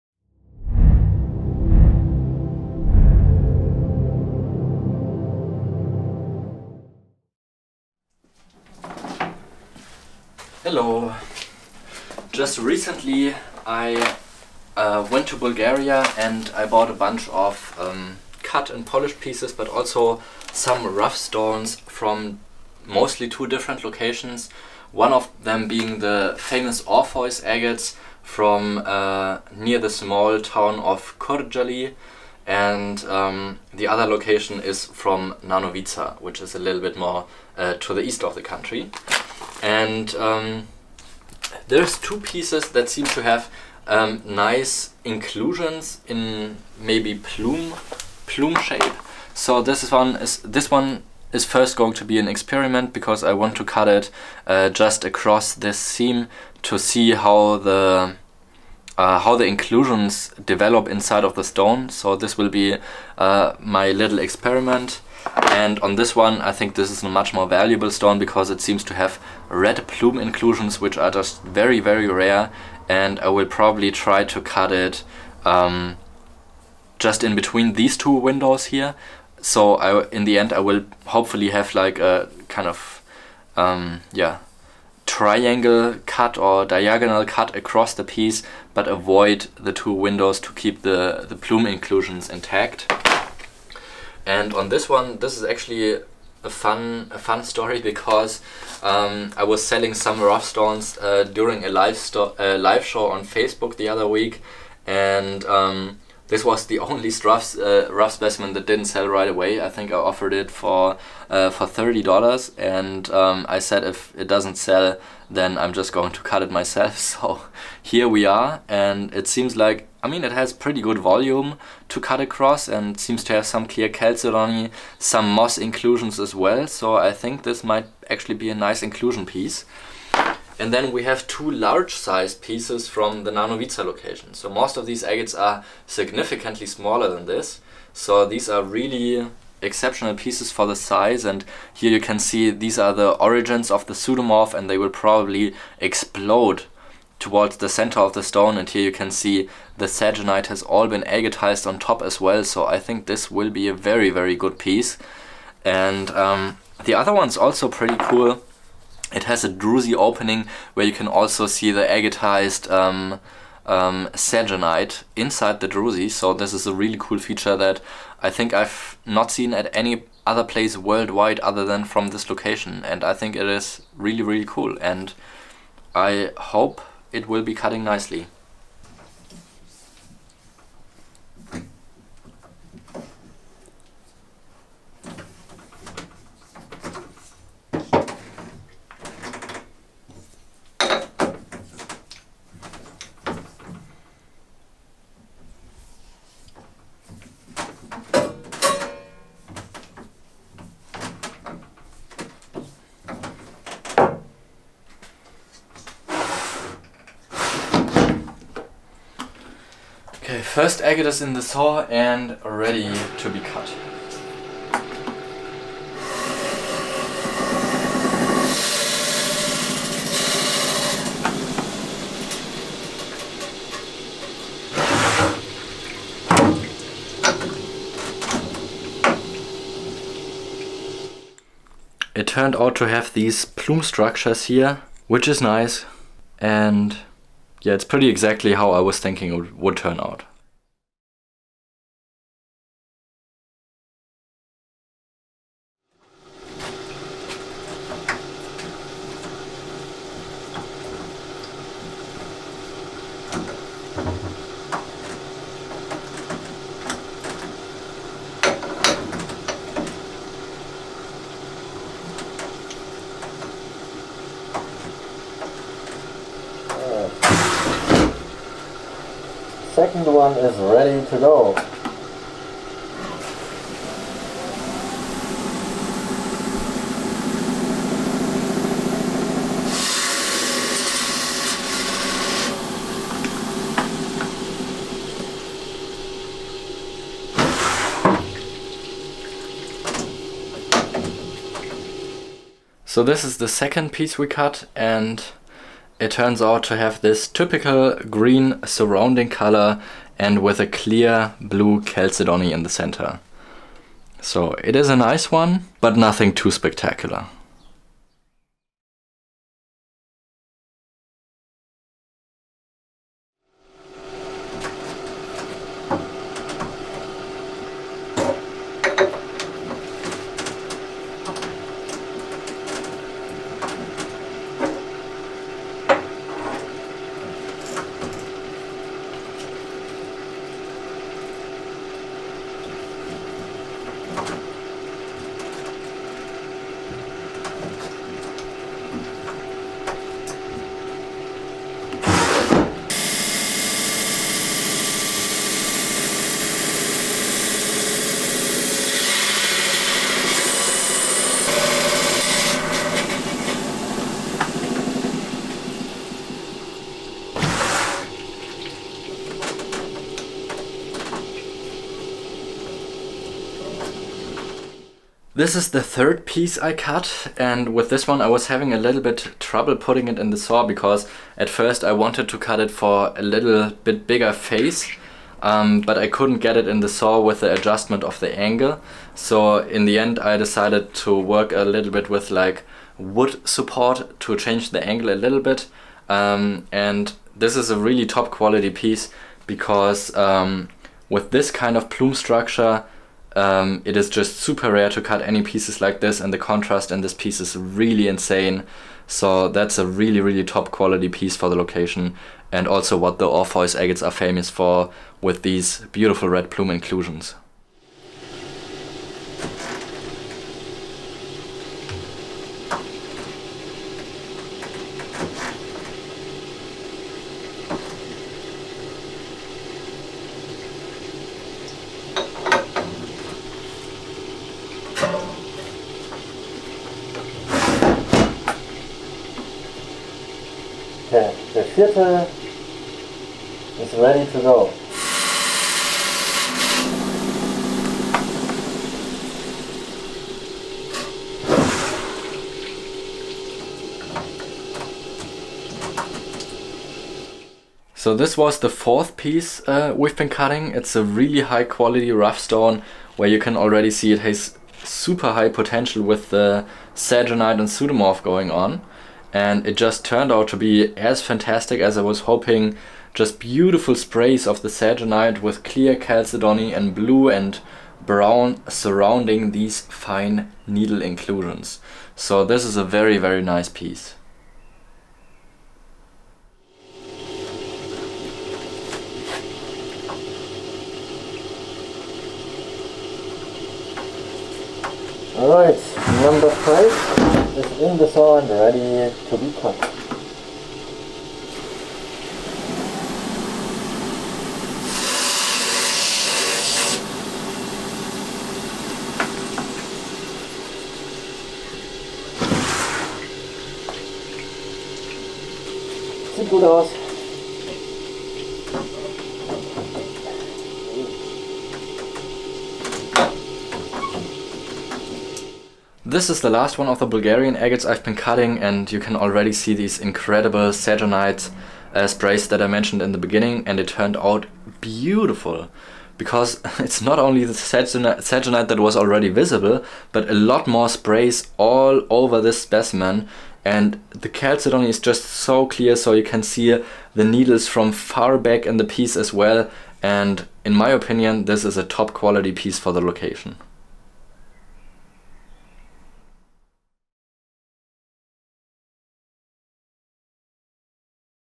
Let's CUT 5 very different Bulgarian AGATES! -4K-
Let's cut 5 very different Bulgarian agates:

Just recently I was in Bulgaria and bought some raw agate stones.
In this video, I will take you with me on my thoughtprocess of how I cut 5 very different Bulgarian agates.
Cutting agates is not as easy as it may seem, a lot of thought goes into it, and sometimes difficult decisions need to be made. I explain how to place a cut to avoid windows in the husk of an agate and many more details that are helpful to know, if you want to cut agates.
Agates are truly amazing pieces of art, made by mother nature, but there is only one way to reveal their beauty: Cutting the agate.

Joshua Ritter
Agatebay